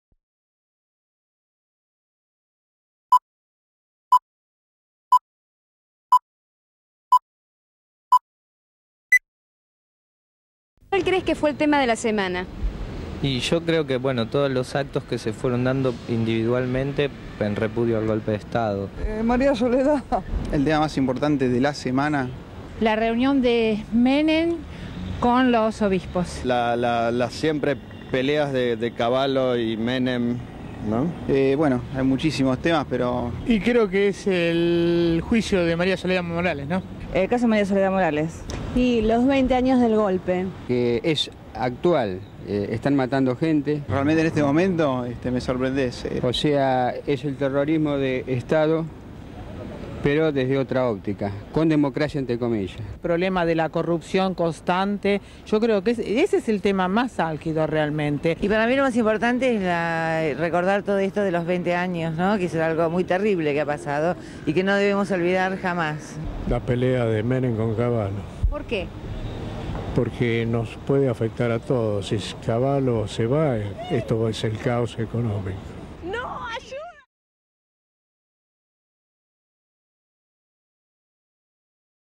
DiFilm - Encuesta en Plaza de Mayo tema de la semana (1996)
La gente responde: "Los actos en repudio del 20º aniversario del Golpe de Estado"; "La reunión de Menem con los obispos"; "La pelea de Menem con Cavallo"; "El juicio de Maria Soledad Morales"; "La corrupción".
Fecha: 25/3/1996
Duración: 1 minuto 6 segundos
Código: BG-0393